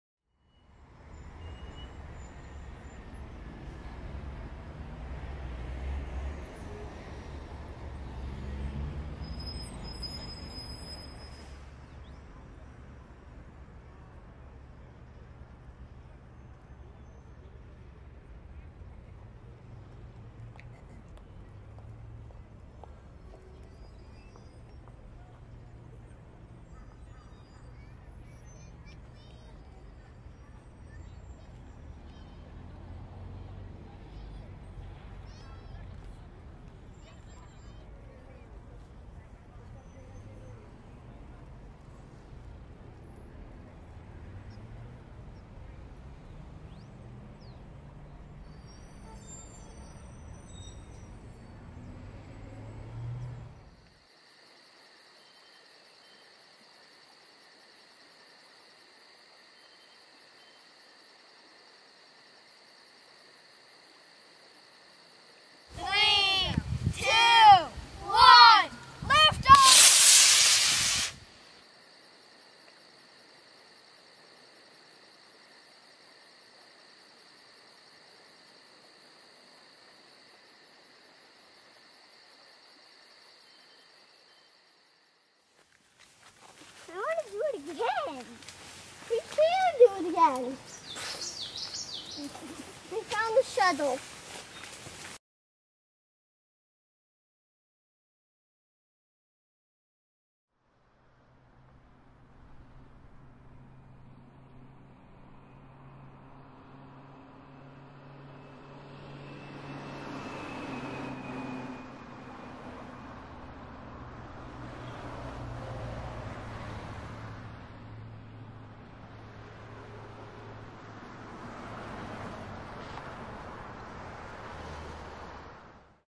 High Speed Video Summer
My camera has a high speed video option, so this summer we had some silly fun with it.  Letting your kids play with mouse traps and rockets is fun!